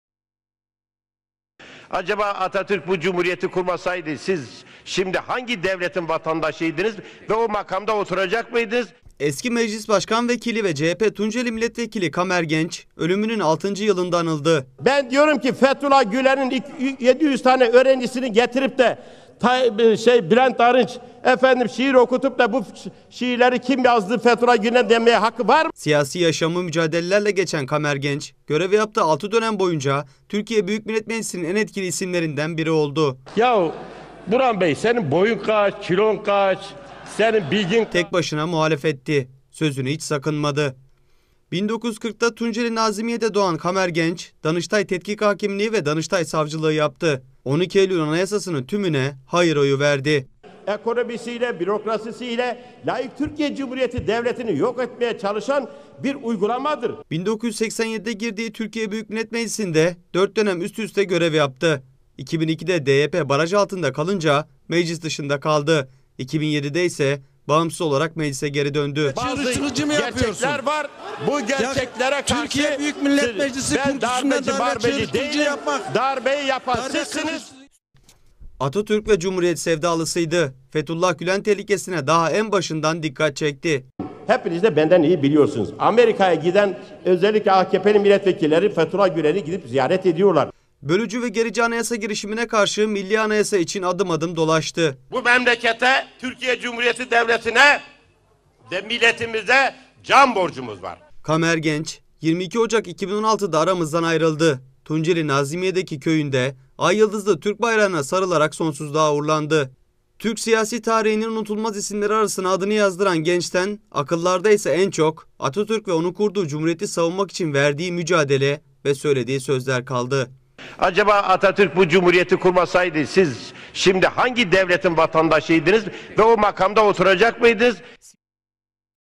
Kamer Genç vefatının 6. yılında unutulmadı
Türk siyaset tarihinden çok sayıda önemli isim geldi geçti. Kimi icraatları, kimiyse yapmadıkları ile akıllarda kaldı. Ama öyle biri vardı ki;  Gazi Meclis'te aynı görüşü paylaşmasalar bile herkesin sevgisini kazandı.  Türk siyasetine adını "Tek kişilik ordu" olarak yazdıran Kamer Genç, bundan tam 6 yıl önce aramızdan ayrıldı.